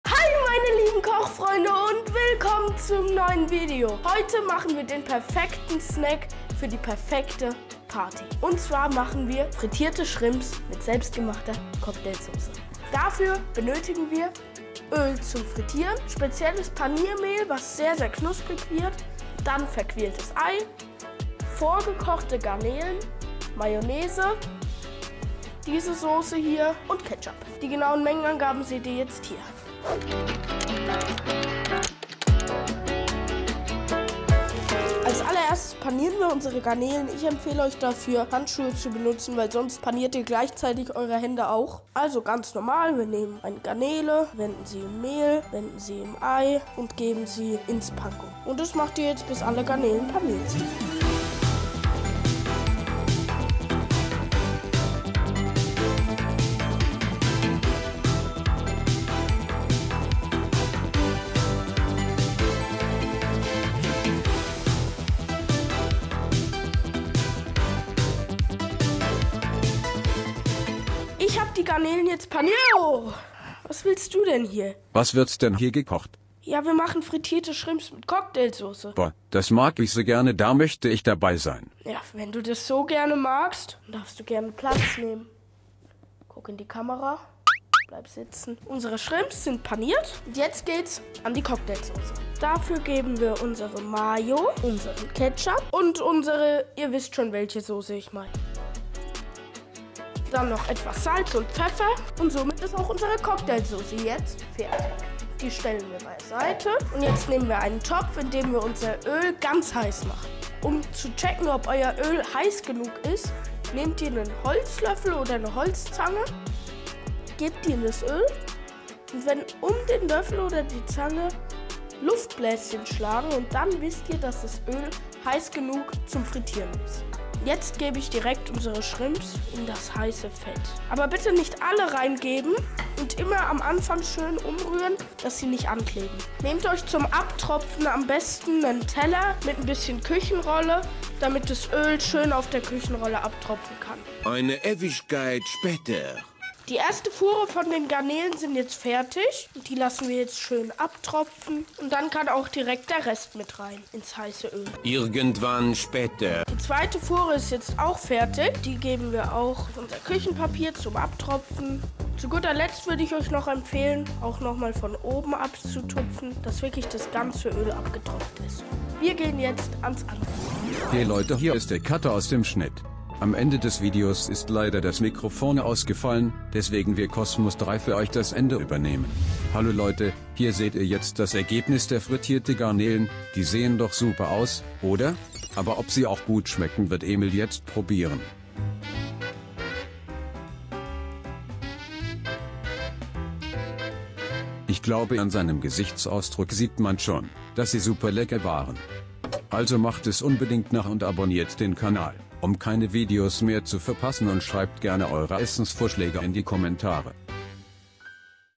Der BESTE Partysnack - frittierte Shrimps mit Cocktailsauce
Rezept:
Für 3 - 4 Personen:

 250g vorgekochte Garnelen
- Panko Paniermehl
- Mehl
- Ei

Cocktailsoße:
- 250g Mayo
- 6 EL Ketchup
- etwas Worchestersauce
- Salz & Pfeffer